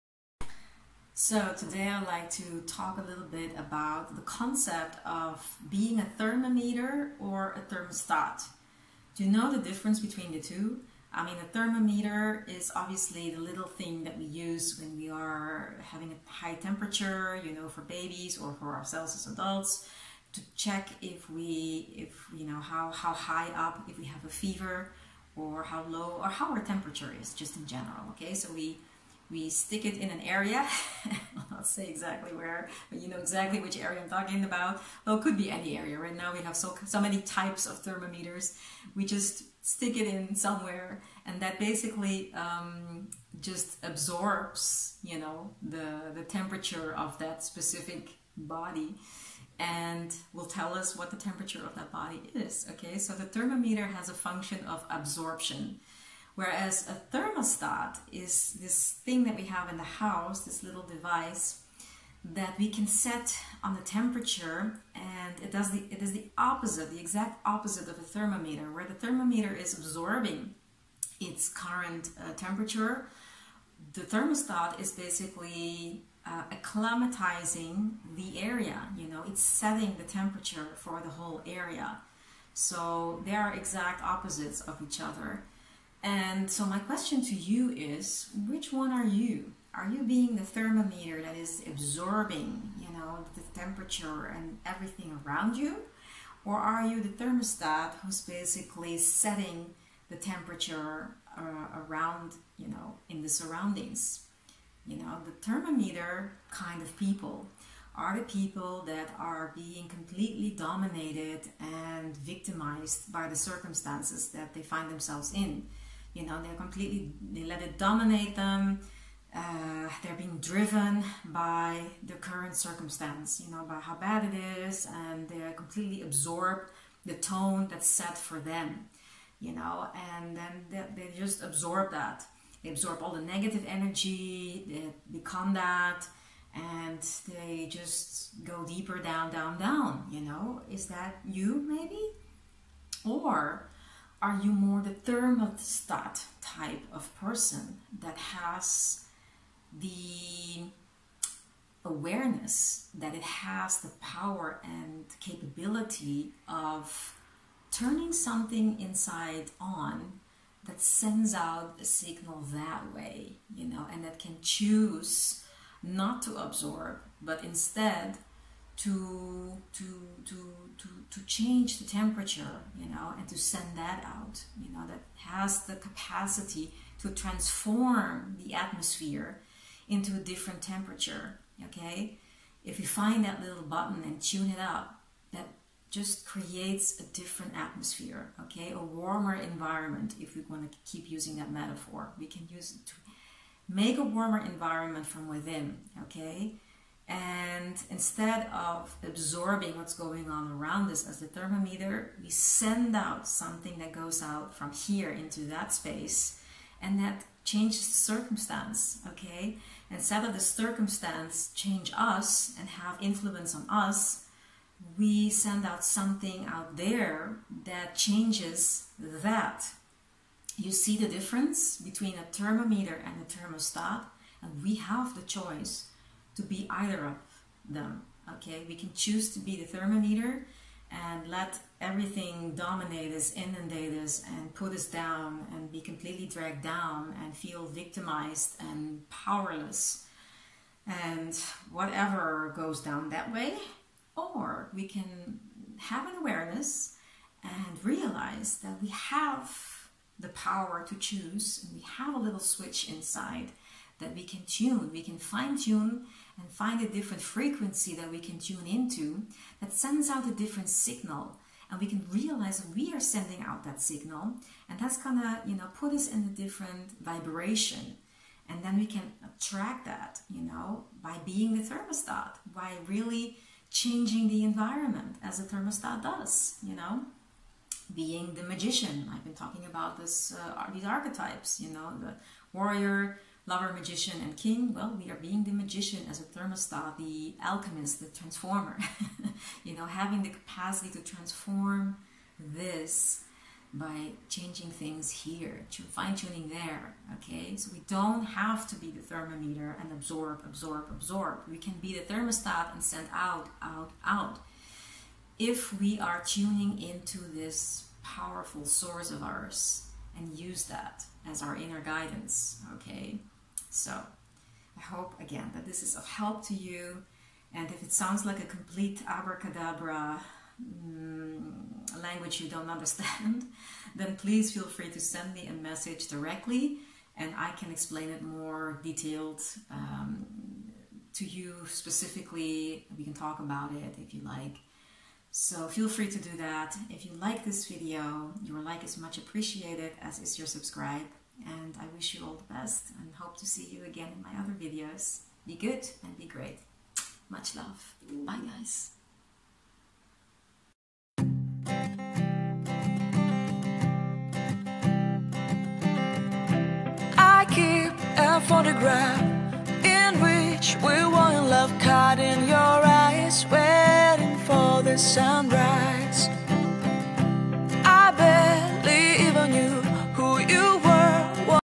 ARE YOU AN INFLUENCER OR DO YOU GET INFLUENCED? ✨🌞
Are you a person that uses your Capicity to uplift others or at least keep your own Light on? Or do you let yourself get dragged down into Sorrow and Darkness by others?